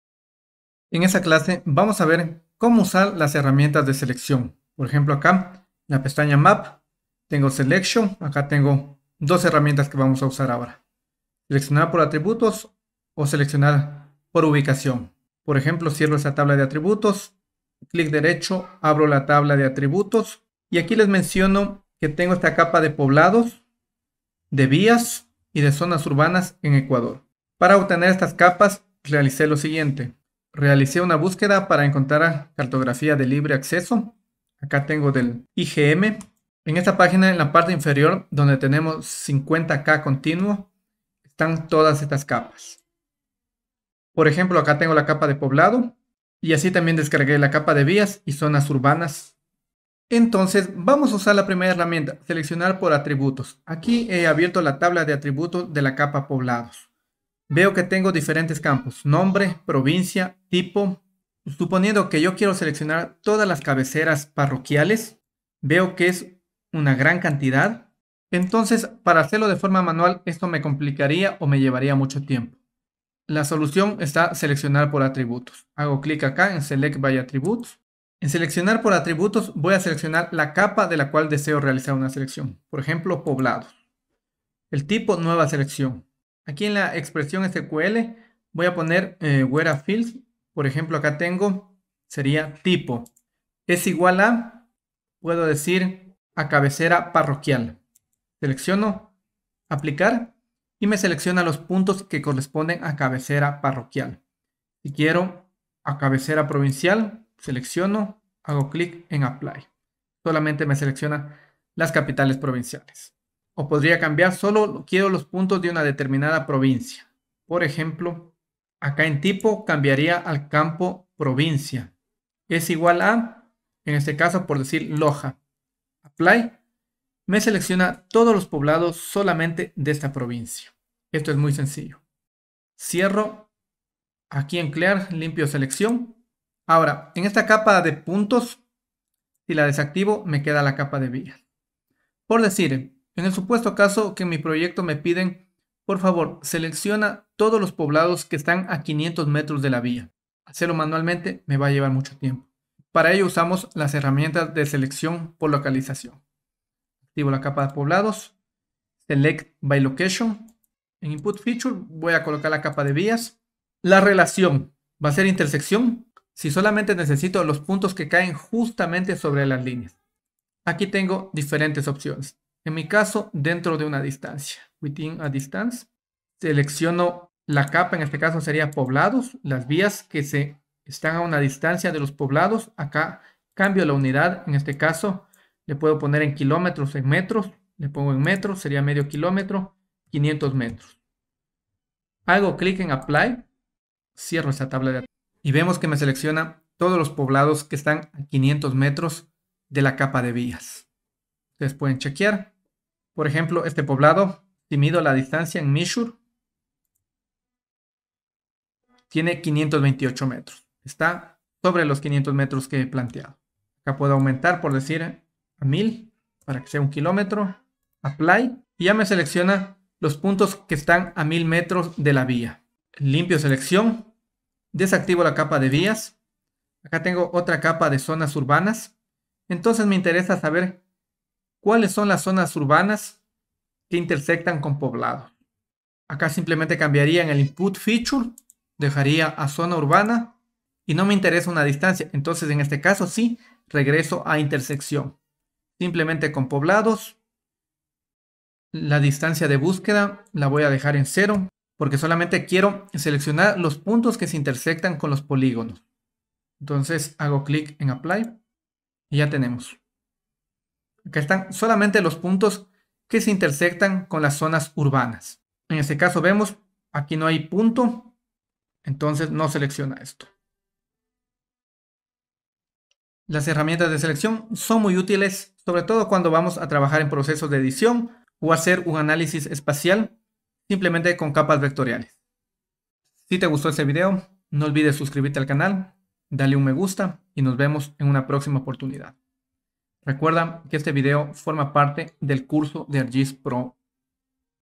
Herramientas de selección en ArcGIS Pro
Tutorial básico para comprender el uso de las herramientas de selección Select By Attributes y Select By Location usando ArcGIS Pro.